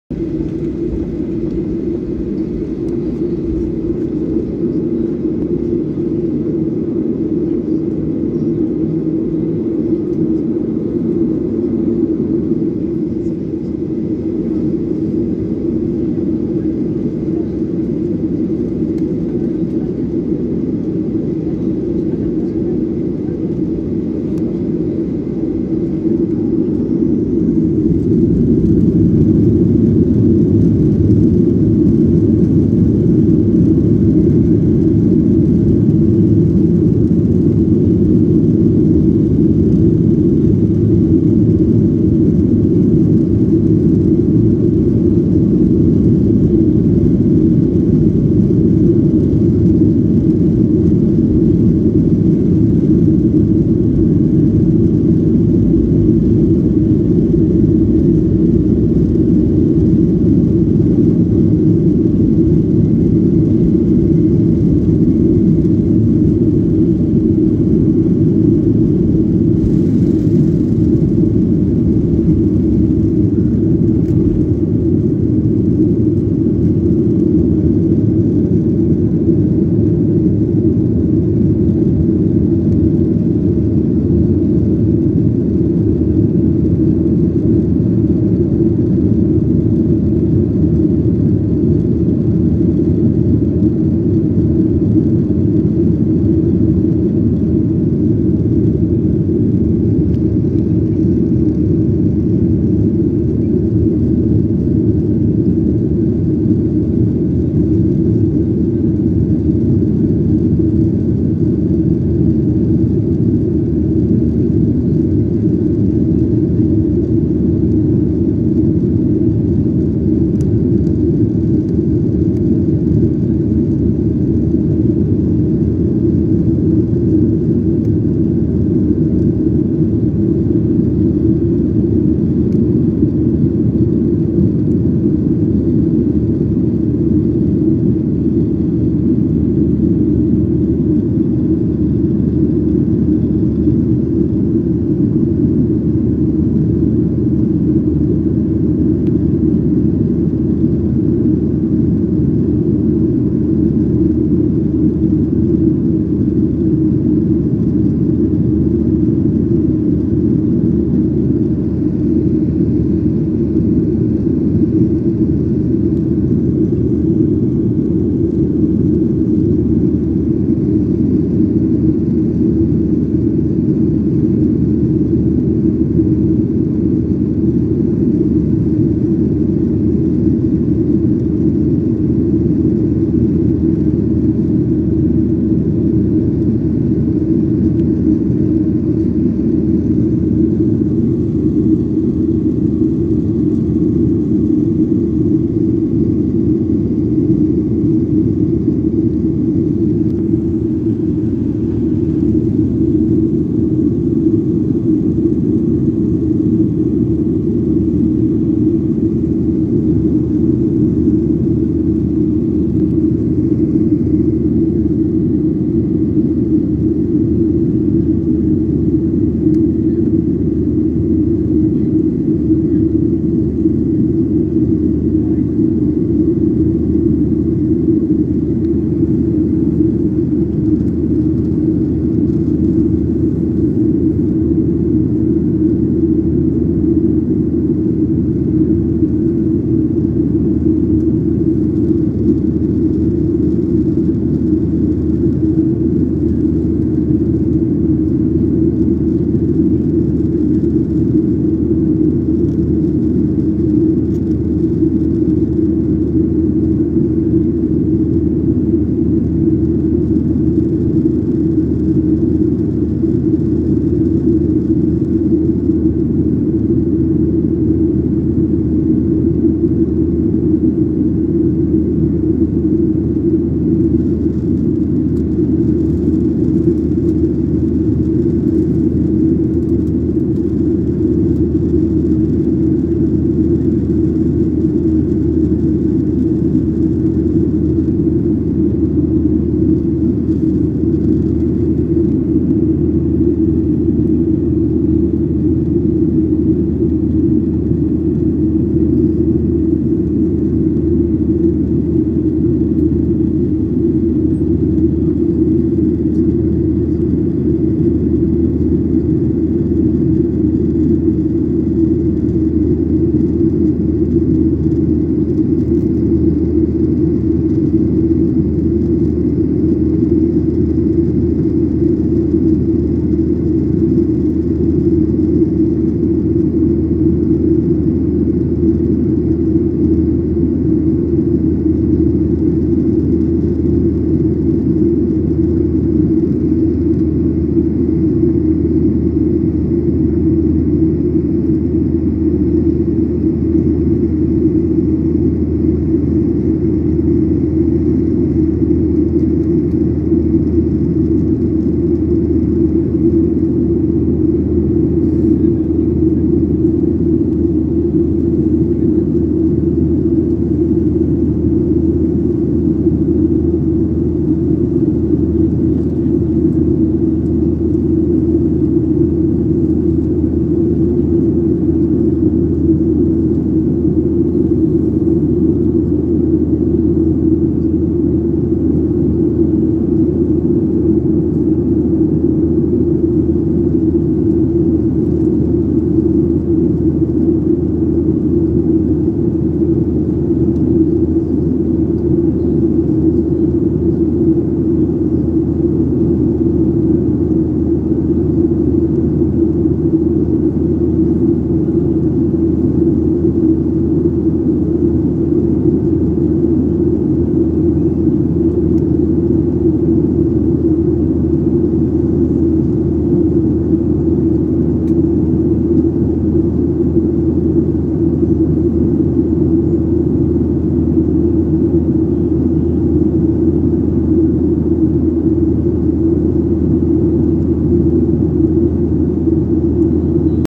Despegue Aeropuerto Internacional Ezeiza - Aeropuerto Internacional de Ushuaia - Flybondi.
flybondi, boeing 737-800, LV-HKN. Rumbo Aeropuerto Internacional de Ushuaia. 31/03/22